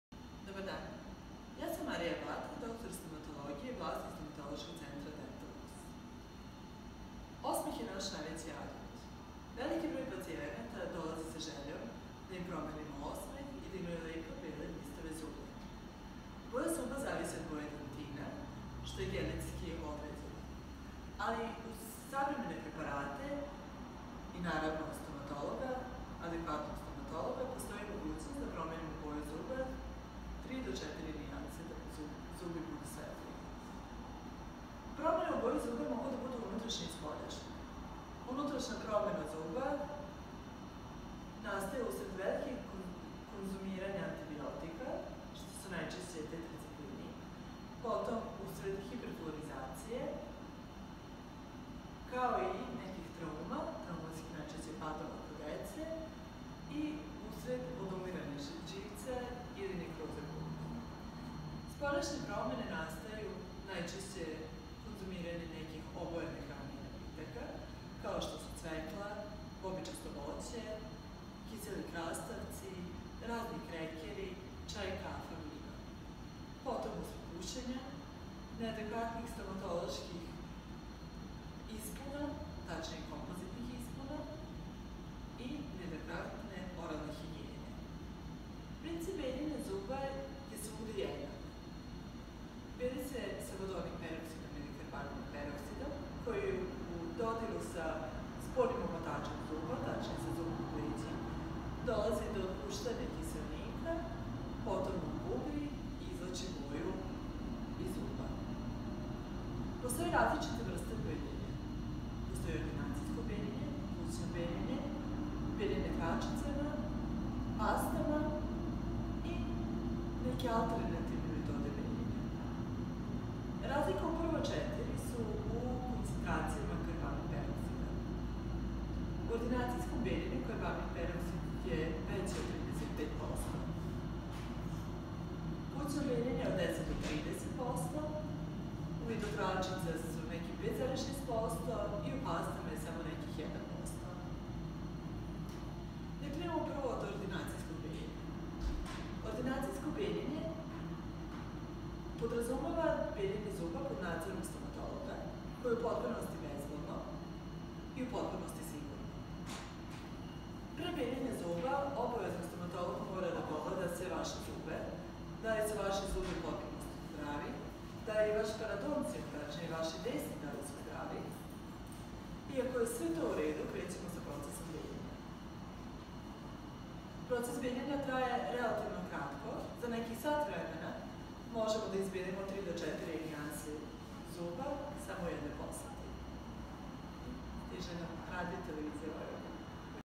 Beljenje zuba dr Batak
Razlozi za promenu boje zuba, podela nacina bejenja zuba, Dentalux Batak Novi Sad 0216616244